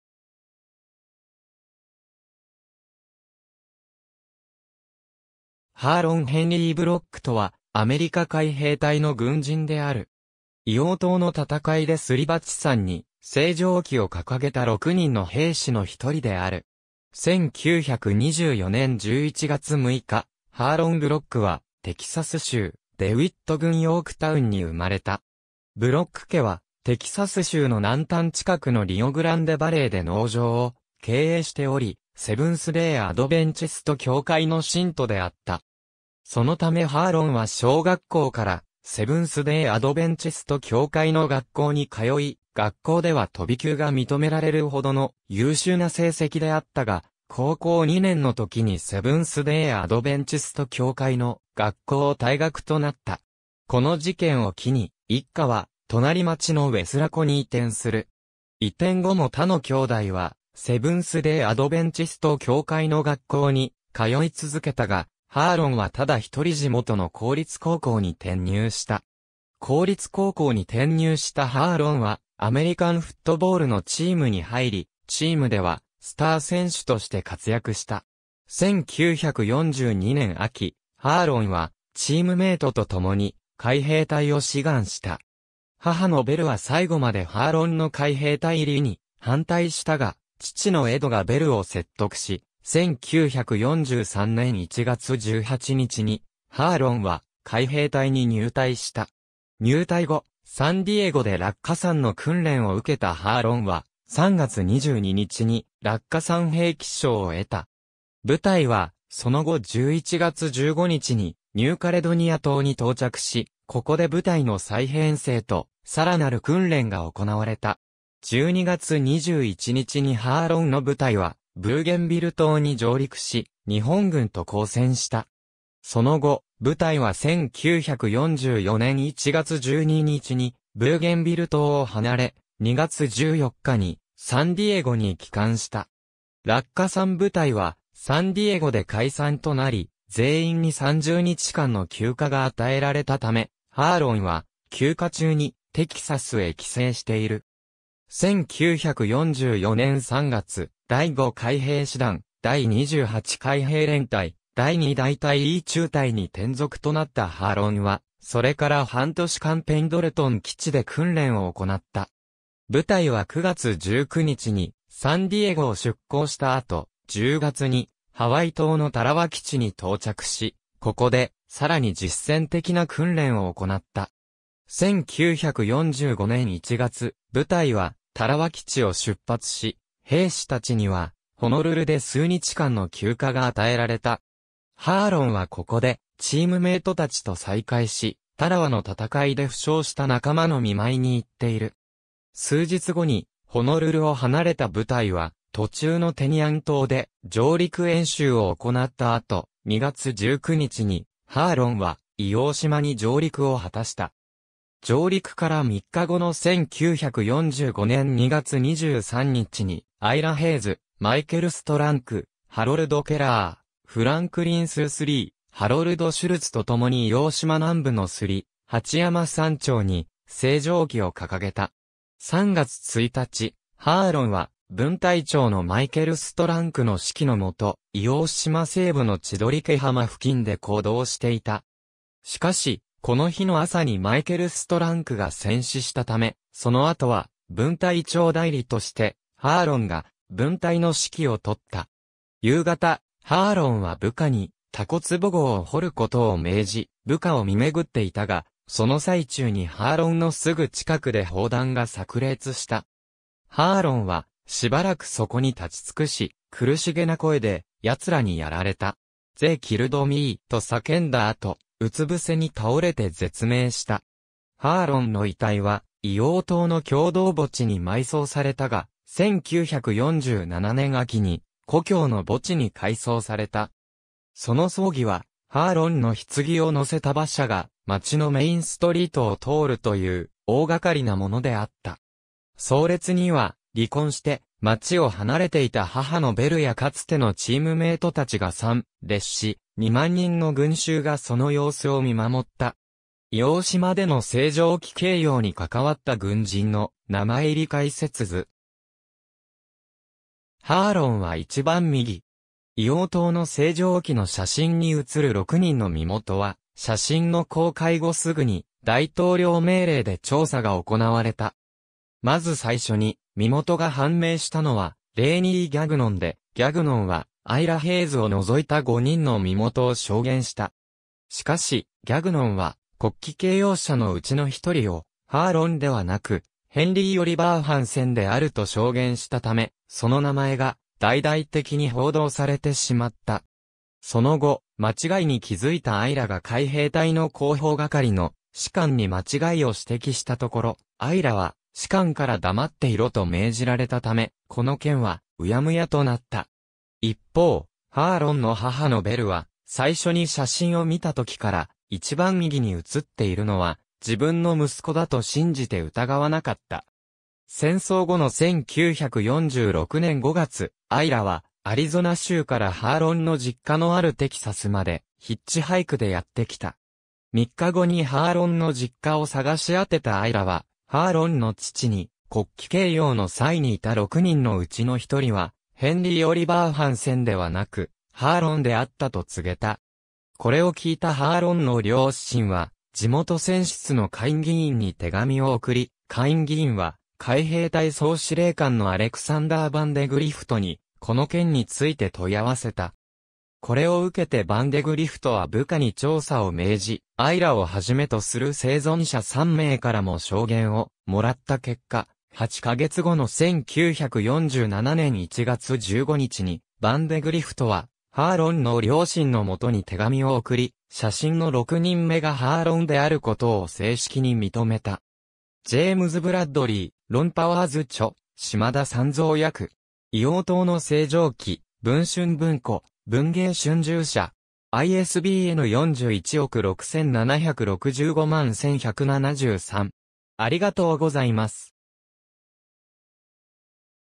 ハーロン・ブロック
ハーロン・ヘンリー・ブロック(英語: Harlon Henry Block、1924年11月6日 - 1945年3月1日)とは、アメリカ海兵隊の軍人である。
硫黄島の戦いで摺鉢山に星条旗を掲げた6人の兵士の1人である。
1924年11月6日、ハーロン・ブロック（以下「ハーロン」と記す）はテキサス州デウィット郡ヨークタウンに生まれた。
ブロック家はテキサス州の南端近くのリオ・グランデ・バレーで農場を経営しており、セブンスデー・アドベンチスト教会（第7日安息日再臨派）の信徒であった。
そのためハーロンは小学校からセブンスデー・アドベンチスト教会の学校に通い、学校では飛び級が認められるほどの優秀な成績であったが、高校2年の時にセブンスデー・アドベンチスト教会の学校を退学となった。
この事件を機に一家は隣町のウェスラコに移転する。
移転後も他の兄弟はセブンスデー・アドベンチスト教会の学校に通い続けたが、ハーロンはただ一人地元の公立高校に転入した。
公立高校に転入したハーロンはアメリカンフットボールのチームに入り、チームではスター選手として活躍した。
1942年秋、ハーロンはチームメイトとともに海兵隊を志願した。
母のベルは最後までハーロンの海兵隊入りに反対したが、父のエドがベルを説得し、1943年1月18日にハーロンは海兵隊に入隊した。
入隊後、サン・ディエゴで落下傘の訓練を受けたハーロンは、3月22日に落下傘兵記章を得た。
部隊はその後11月15日にニューカレドニア島に到着し、ここで部隊の再編成と更なる訓練が行われた。
12月21日にハーロンの部隊はブーゲンビル島に上陸し、日本軍と交戦した。
その後、部隊は1944年1月12日にブーゲンビル島を離れ、2月14日にサン・ディエゴに帰還した。
落下傘部隊はサン・ディエゴで解散となり、全員に30日間の休暇が与えられたため、ハーロンは休暇中にテキサスへ帰省している。
1944年3月、第5海兵師団第28海兵連隊第2大隊E中隊に転属となったハーロンは、それから半年間ペンドルトン基地で訓練を行った。
部隊は9月19日にサン・ディエゴを出港した後、10月にハワイ島のタラワ基地に到着し、ここで更に実戦的な訓練を行った。
1945年1月、部隊はタラワ基地を出発し、兵士たちにはホノルルで数日間の休暇が与えられた。
ハーロンはここでチームメイト達と再会し、タラワの戦いで負傷した仲間の見舞いに行っている。
数日後にホノルルを離れた部隊は、途中のテニアン島で上陸演習を行った後、2月19日にハーロンは硫黄島に上陸を果たした。
上陸から3日後の1945年2月23日にアイラ・ヘイズ、マイケル・ストランク、ハロルド・ケラー、フランクリン・スースリー、ハロルド・シュルツと共に硫黄島南部の摺鉢山山頂に星条旗を掲げた。
3月1日、ハーロンは分隊長のマイケル・ストランクの指揮のもと、硫黄島西部の千鳥ケ浜附近で行動していた。
しかし、この日の朝にマイケル・ストランクが戦死したため、その後は分隊長代理としてハーロンが分隊の指揮をとった。
夕方、ハーロンは部下に蛸壺壕を掘ることを命じ、部下を見廻っていたが、その最中にハーロンのすぐ近くで砲弾が炸裂した。
ハーロンはしばらくそこに立ちつくし、苦しげな声で「やつらにやられた！」"They killed me!"と叫んだ後、うつ伏せに倒れて絶命した。
ハーロンの遺体は硫黄島の共同墓地に埋葬されたが、1947年秋に故郷の墓地に改葬された。
その葬儀は、ハーロンの棺を乗せた馬車が街のメインストリートを通るという大がかりなものであった。
葬列には、離婚して街を離れていた母のベルやかつてのチームメイトたちが参列し、2万人の群衆がその様子を見守った。
硫黄島での星条旗掲揚に関わった軍人の名前入り解説図。
ハーロンは一番右。
硫黄島の星条旗の写真に写る6人の身元は、写真の公開後すぐに大統領命令で調査が行われた。
まず最初に身元が判明したのはレイニー・ギャグノン（以下「ギャグノン」と記す）で、ギャグノンはアイラ・ヘイズ（以下「アイラ」と記す）を除いた5人の身元を証言した。
しかし、ギャグノンは国旗掲揚者のうちの1人をハーロンではなく、ヘンリー・オリバー・ハンセン（Henry Oliver Hansen）であると証言したため、その名前が大々的に報道されてしまっ...